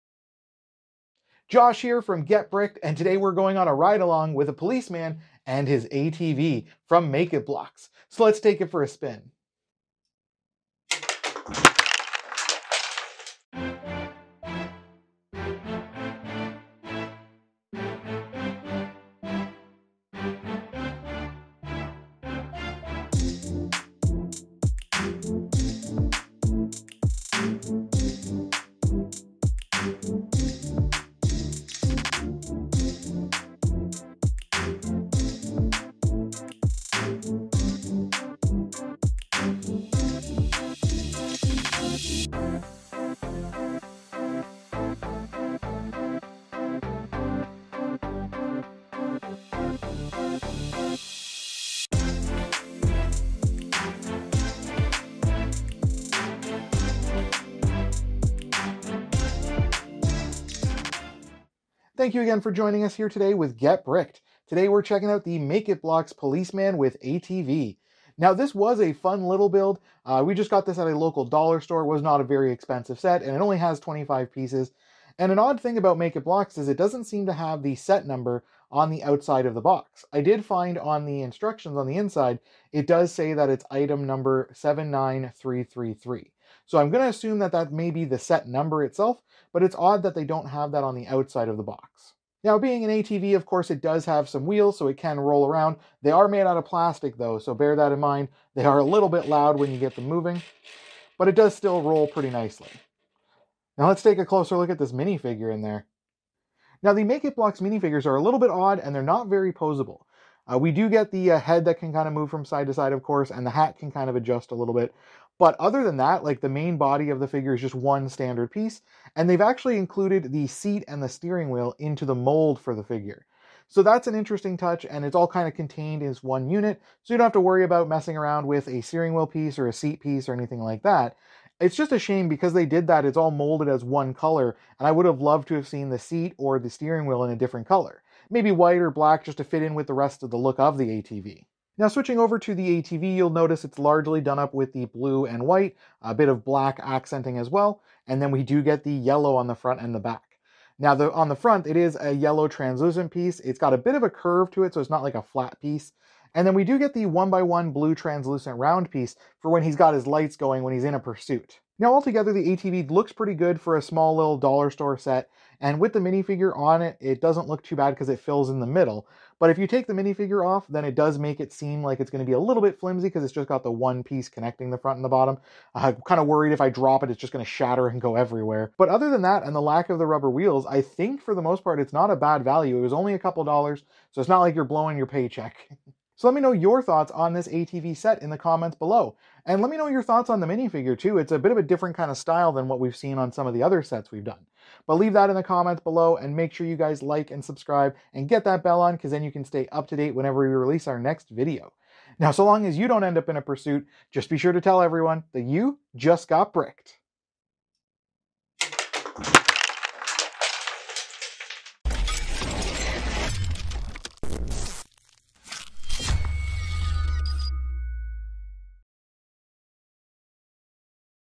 Buildable ATV from a Dollar Store? | Make-It Blocks | Policeman with ATV | Item 79333
Stop in the name of the law! Join us as we check out a Policeman with ATV from Make-It Blocks. Set/Item 79333

Gear Used:
Rode Wireless Go II Dual Channel Mic
Turntable Display
Studio Lights